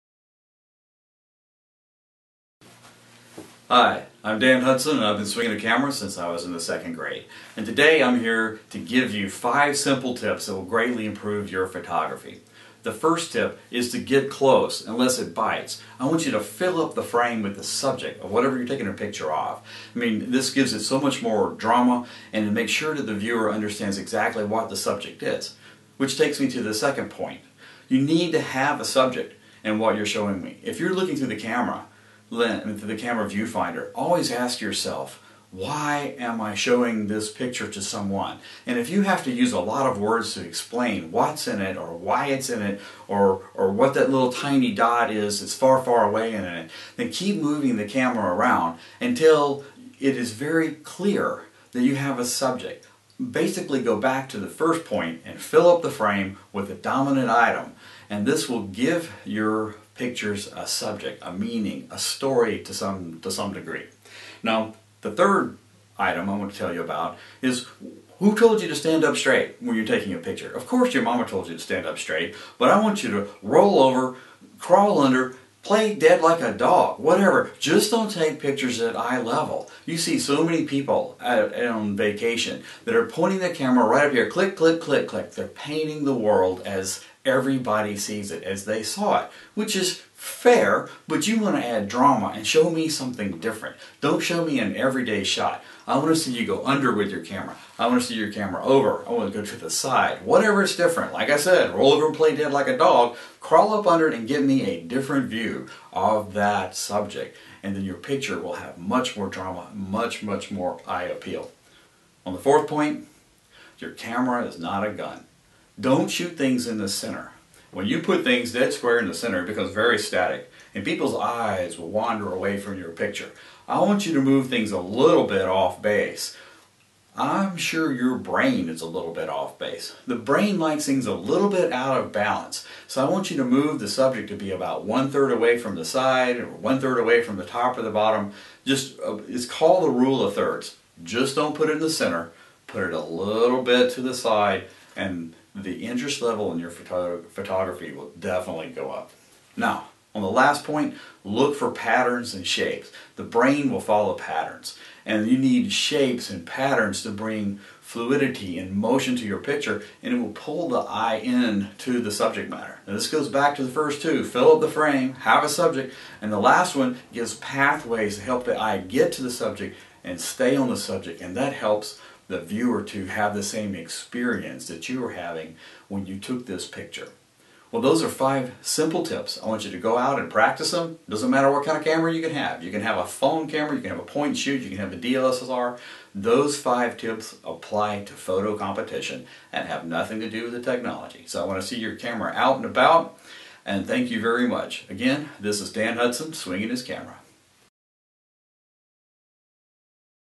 Dan's 5 Photo Tips
Five basic tips to give your pics a lot more 'pop'